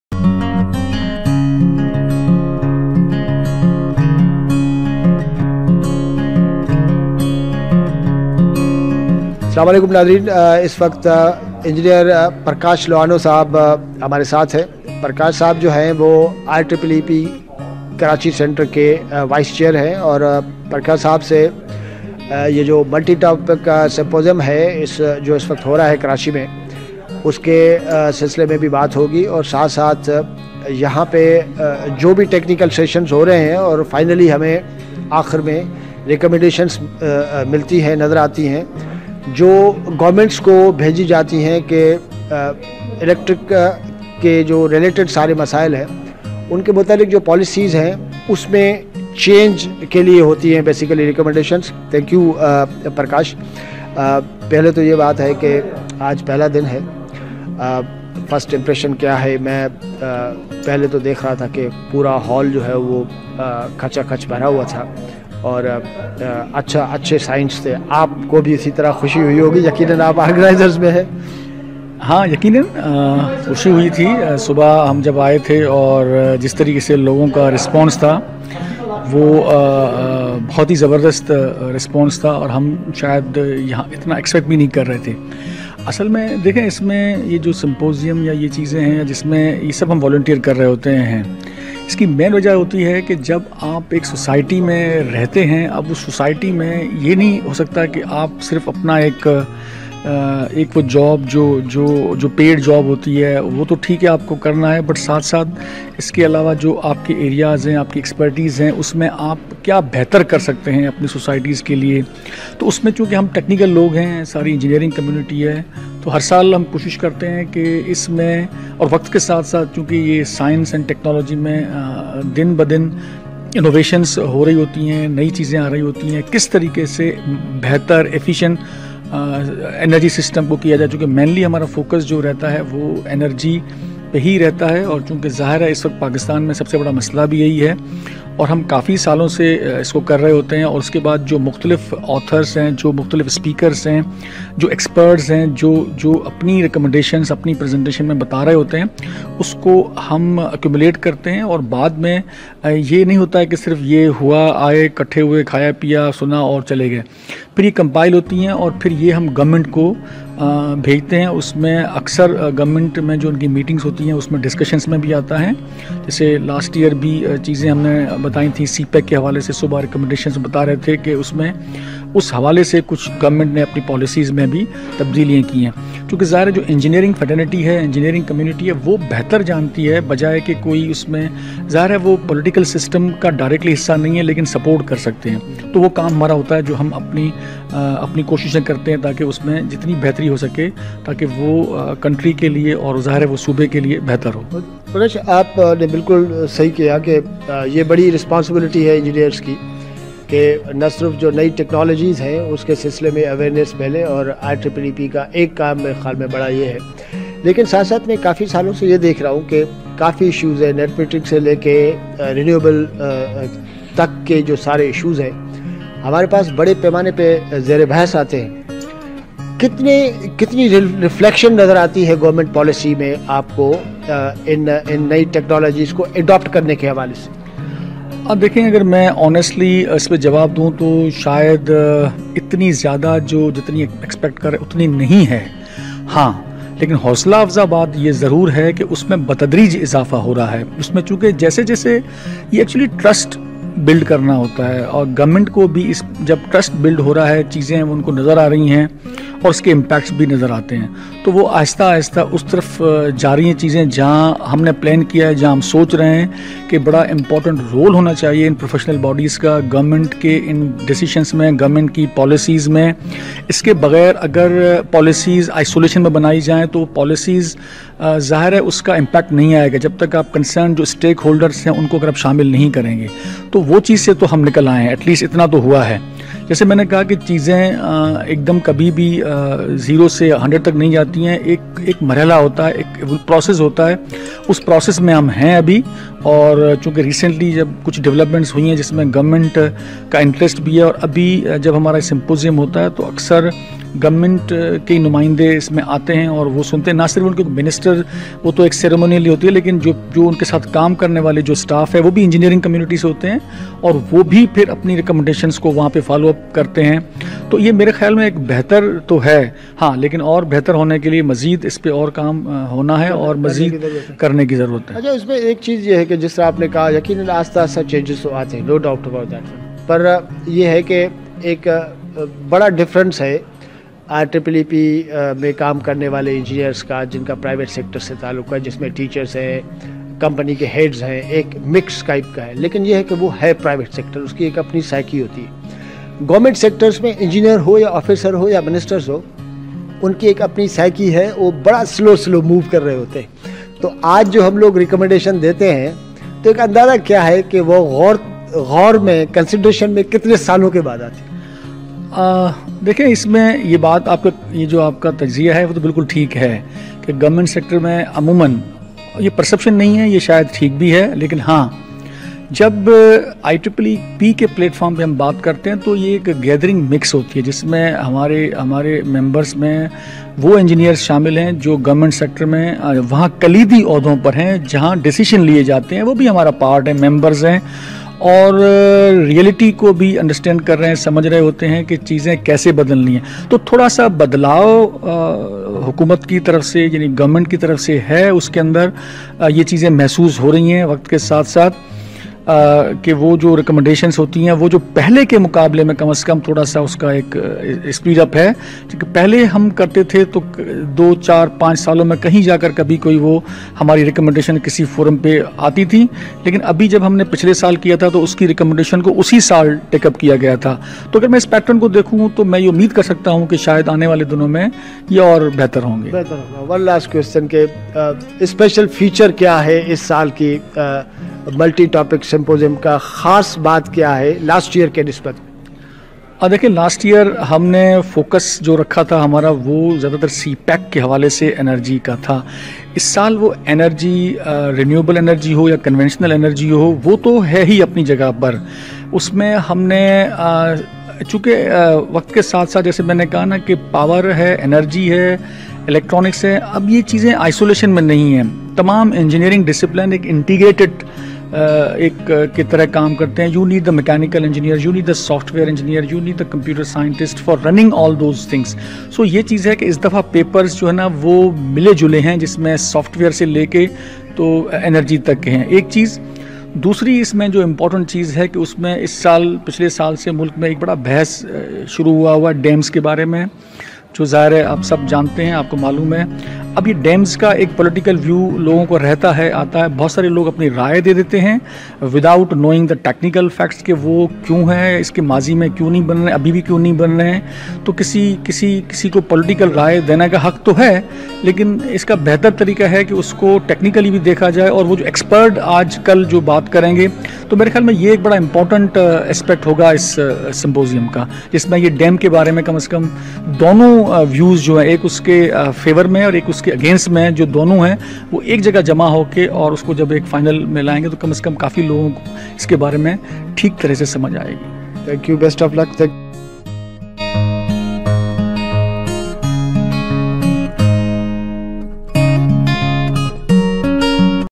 Engineering Review | IEEEP | 34th Multi-Topic Symposium | Engr. Parkash Lohana Talks to ER
Engineering Review | 34th Multi-Topic Symposium | Engr. Parkash Lohana Talks to ER
Interview By: Manzoor Shaikh